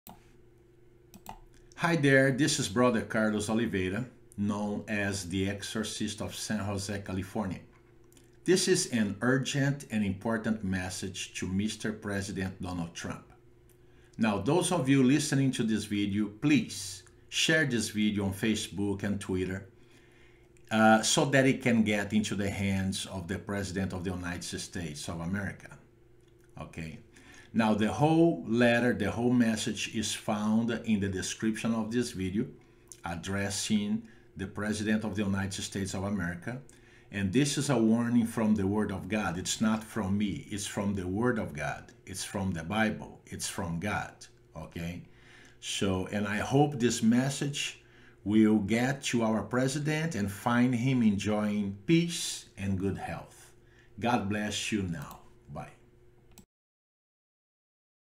AN URGENT AND IMPORTANT MESSAGE TO MR. PRESIDENT DONALD TRUMP. It's found in the description below.
Dear Mr. President Donald Trump, please, read the following:

***Exodus 22:21, “You shall not wrong or oppress a resident alien..."

***Malachi 3:5
And I will come near you for judgment;
I will be a swift witness...
Against those who turn away an alien—
Because they do not fear Me,”

***Jeremiah 22:3
Do no wrong or violence to the foreigner,..."

Dear president, since your friends, the fat cat evangelicals, have never warned you regarding the Word of God in the matter of illegal aliens (not criminal aliens), I hereby address you, with all the respect you are due, regarding how deeply displeased our Father in Heaven is with you. Because of your ignorance concerning His Word and the fact that you have terrorized the most vulnerable people in America, by persecuting and deporting them in a very inhumane way, the LORD of Hosts has unleashed His anger toward you by punishing you in the area of finances.
The hand of God has come down heavily on you causing you to lose billions of dollars and will continue to lose more as time goes by.
The warning from the LORD is this, as long as you continue to spread fear and terrorize His vulnerable people, illegal immigrants (not criminal illegal immigrants) in America, the LORD of Hosts will continue punishing you until you lose everything, this is the price you will pay. Your reputation may also be on the line.

Sorry, Mr. President, that your friends, the fat cat evangelicals, have never warned you about this truth.

I wish you all the best from the bottom of my heart.

Brother Carlos Oliveira - The Exorcist of San Jose, California.